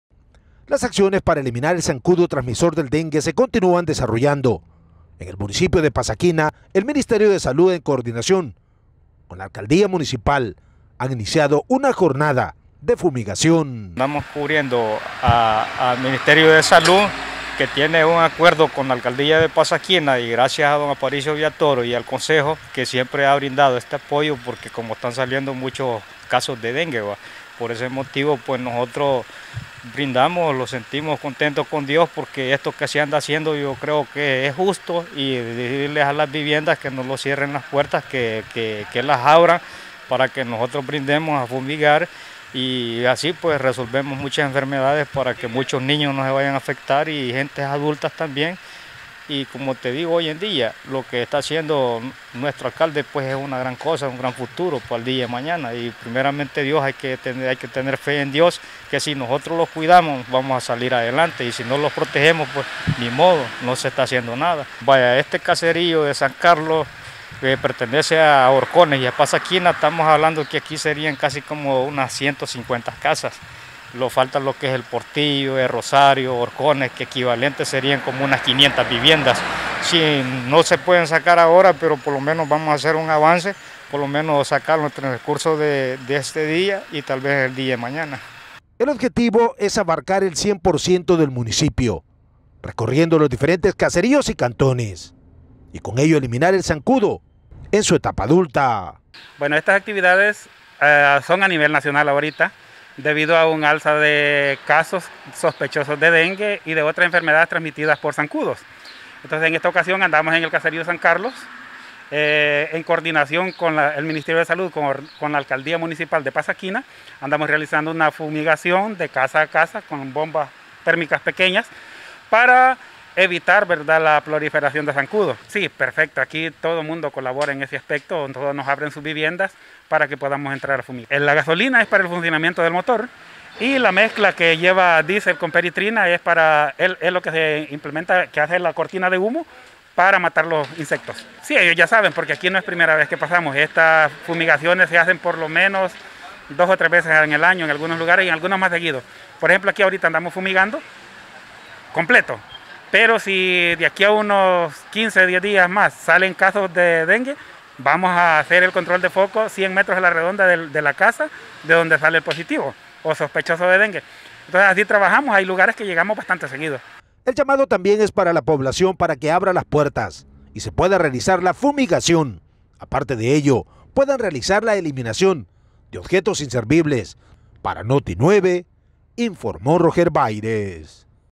NUEVA FUMIGACIÓN, PASAQUINA
El Ministerio de Salud en coordinación con la comuna de Pasaquina, llevaron a cabo una jornada de fumigación el municipio, con el objetivo de bajar el índice larvario y ante el incremento de casos de dengue.